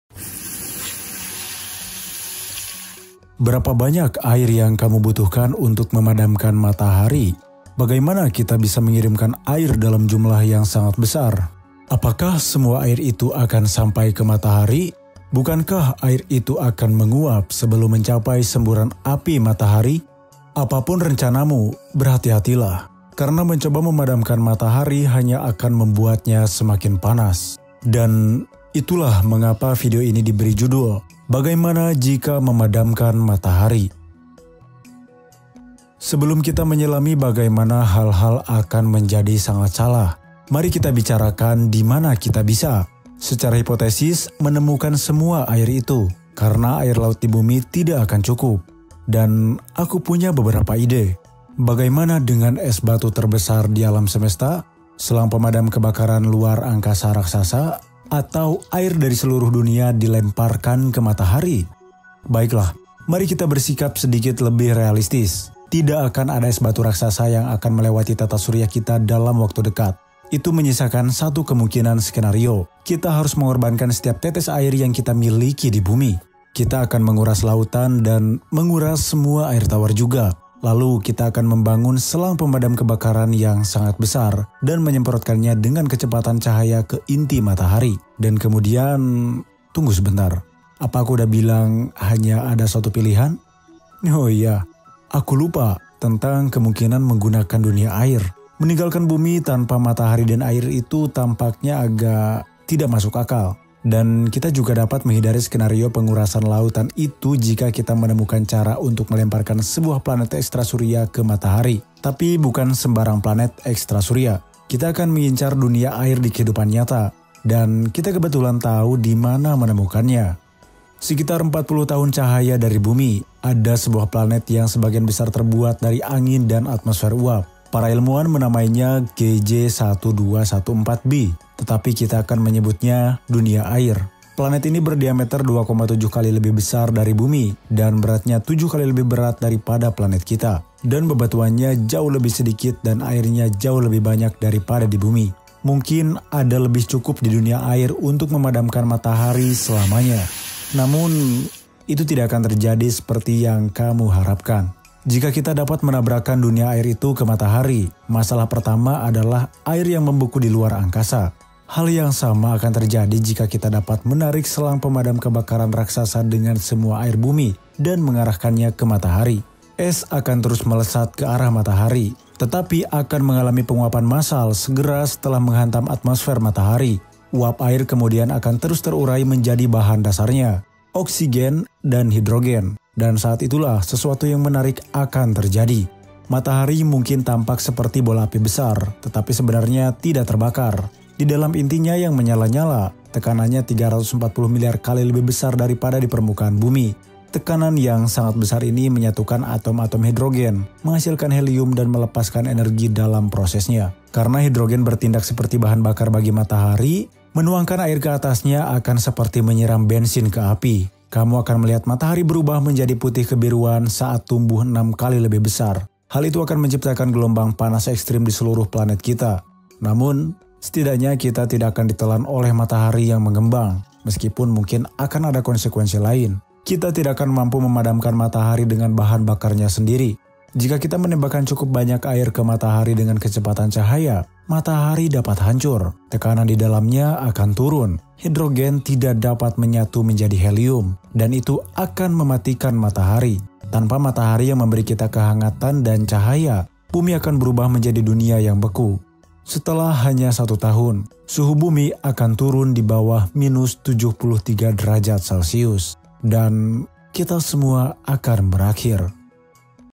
Jika Memadamkan Matahari
Pernahkah terbayang apa yang akan terjadi jika kita benar-benar memadamkan matahari? "Bagaimana Jika Memadamkan Matahari" mengeksplorasi skenario ekstrem ini, di mana matahari sebagai sumber kehidupan tiba-tiba padam. "Bagaimana Jika Memadamkan Matahari" mengajak kita merenungkan dampak dahsyat dari kehilangan cahaya dan panas yang selama ini menjaga keseimbangan di bumi. Dari perubahan iklim yang drastis hingga kehancuran ekosistem, "Bagaimana Jika Memadamkan Matahari" mencoba memprediksi bagaimana manusia dan makhluk hidup lainnya bertahan dalam kegelapan abadi.
Animation Created by Two Sides